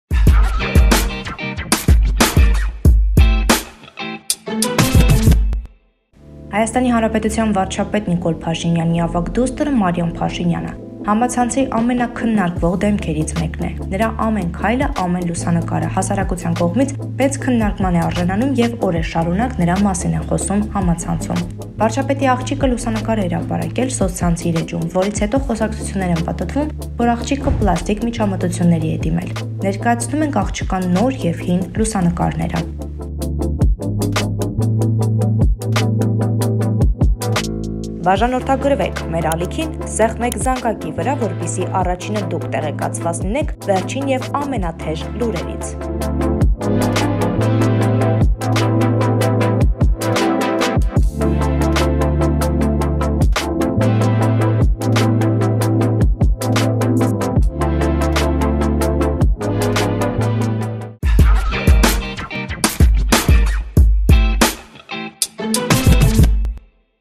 Մարիամ Փաշինյանը պլաստիկ վիրահատությա՞ն է ենթարկվել
ՀՀ վարչապետ Նիկոլ Փաշինյանի ավագ դուստրը Մարիամ Փաշինյանը, համացանցի ամեննաքննարկվող դեմքերից մեկն է, նրա ամեն քայլը, ամեն լուսանկերը հասարակության կողմից մեծ քննարկամն է արժանանում և օրեր շարունակ նրա մասին են խոսում։

Մանրամասն՝ տեսանյութում